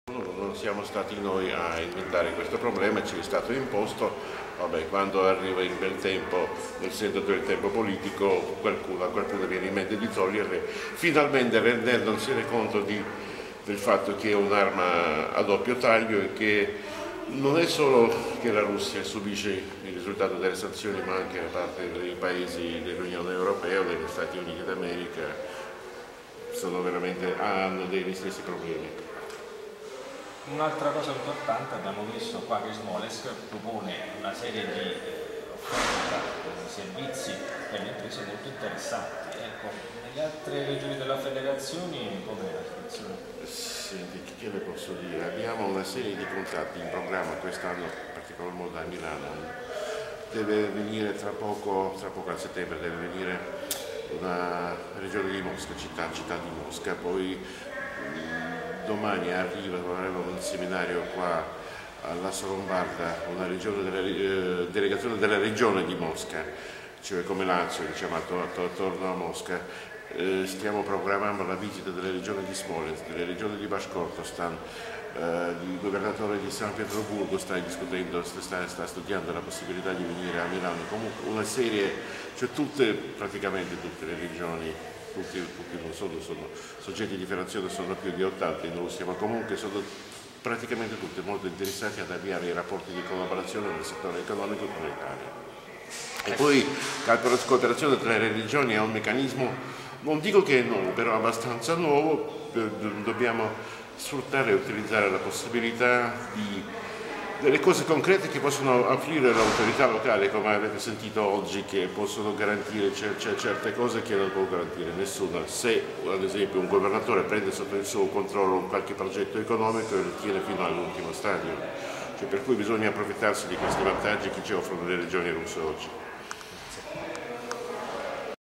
Intervista al Console generale della Federazione Russa, Alexandr Nurizade, sulle sanzioni.
Intervista al Console generale della Federazione Russa a Milano, Alexandr Nurizade, sulle sanzioni alla Russia. In occasione della  presentazione delle iniziative attraverso cui la Regione di Smolensk intende attrarre le aziende italiane. Sala Pirelli dell’ITA (Italian Trade Agency).
Business climate in Smolensk region.
An Institutional and Business Delegation from Smolensk meets the Italian Business community.
Intervista di Francesco Montanino - riprese di Evgeny Utkin